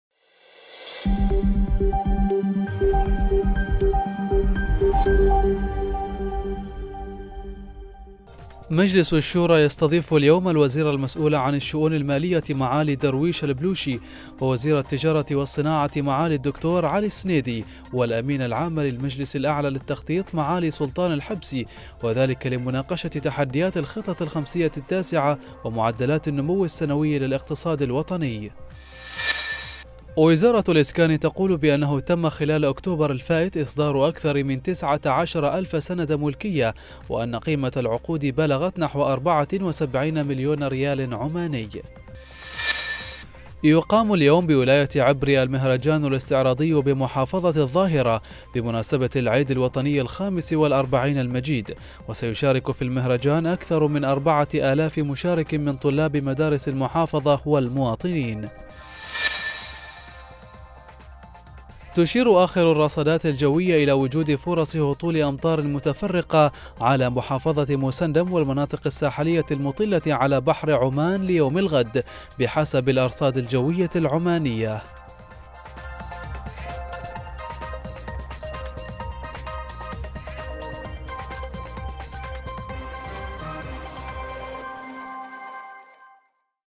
علوم الشبيبة - الاسكان: إصدار 19013 سند ملكية
مسقط: الشورى يستضيف 3 وزراء لمناقشة الخطة الخمسية التاسعة / مسقط: الاسكان: إصدار 19013 سند ملكية / عبري: المهرجان الاستعراضي بمحافظة الظاهرة / مسقط: فرص هطول أمطار متفرقة / للحصول على جميع النشرات حمل تطبيق "واتس علوم"